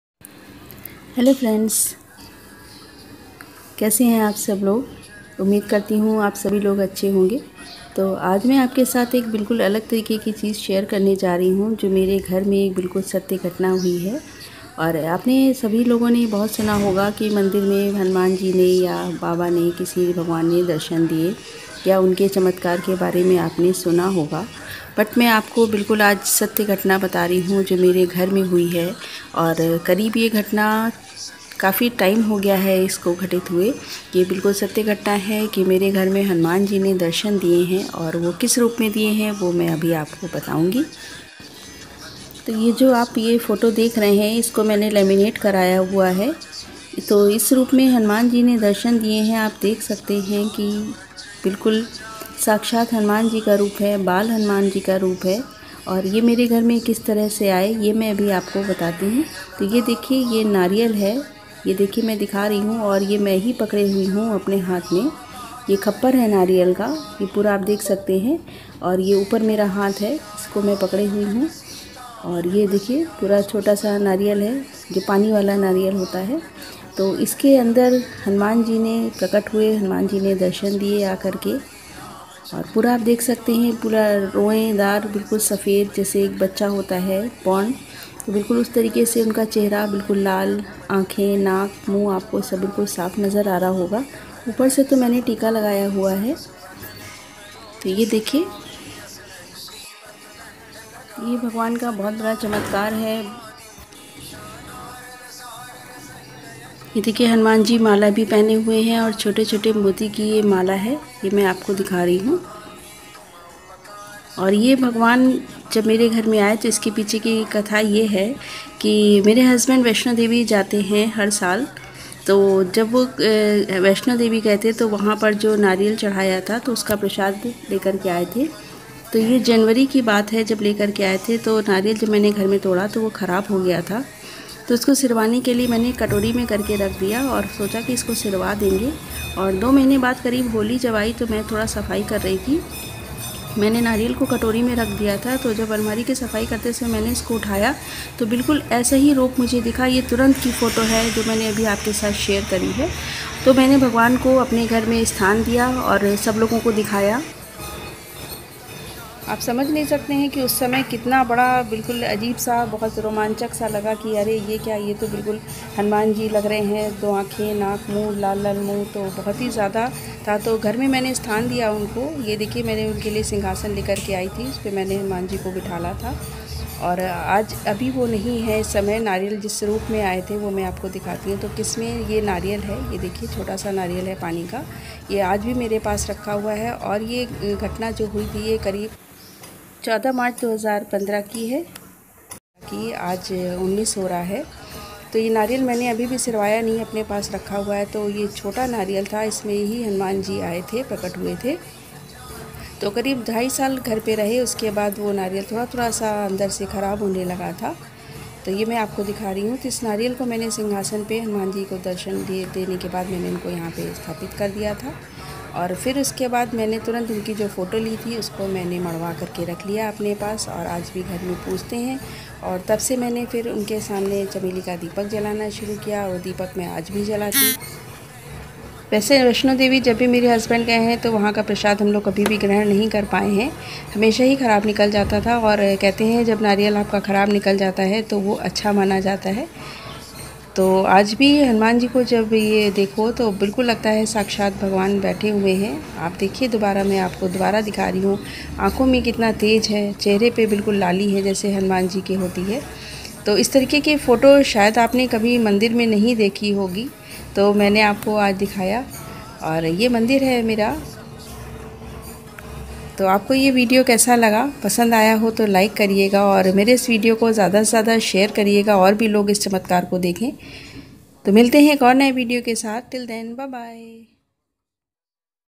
हनुमान जी का चमत्कार /हनुमान दर्शन /miracle of hanuman ji /ढाई वर्षो तक रहे हमारे घर सच्ची घटना
ये एक सत्य घटना है कि हनुमान जी मेरे घर आये और ढाई वर्षो तक हमारे साथ रहे.
This is very true story ...please share this as much you can for hanuman ji darsha#hanumanjikachamatkar#hanumanji

Gunjan 'Diary